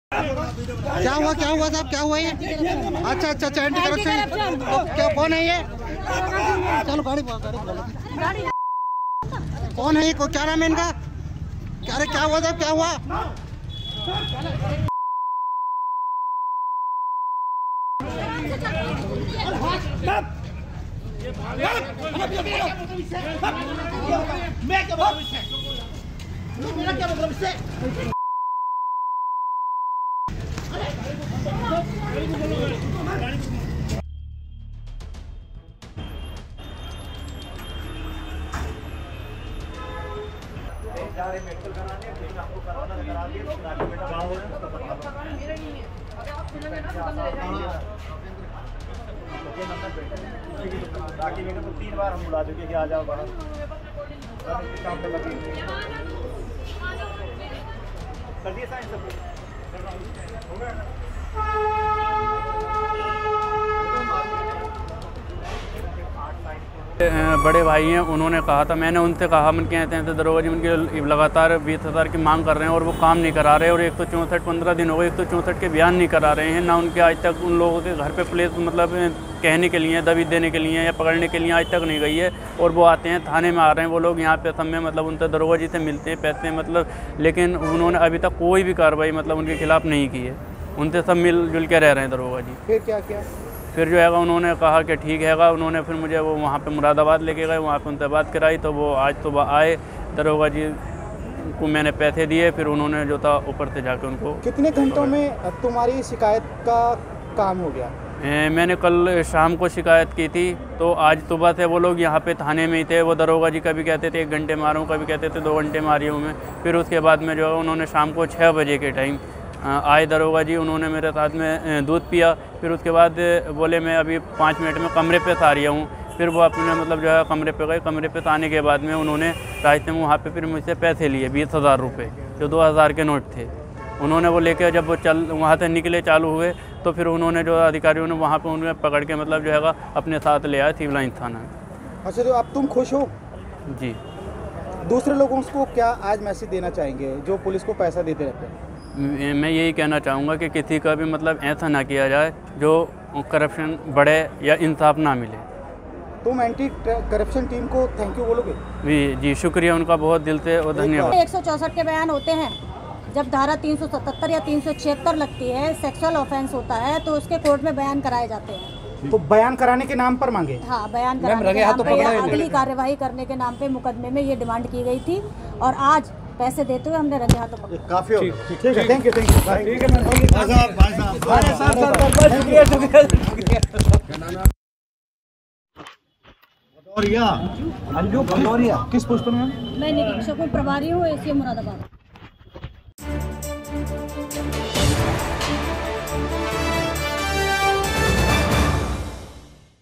एंटी करप्शन टीम ने घूसखोर दरोगा को किया गिरफ्तार | Anti Corruption Team | Crime News | Rampur News
एंटी करप्शन टीम ने घूसखोर दरोगा को किया गिरफ्तार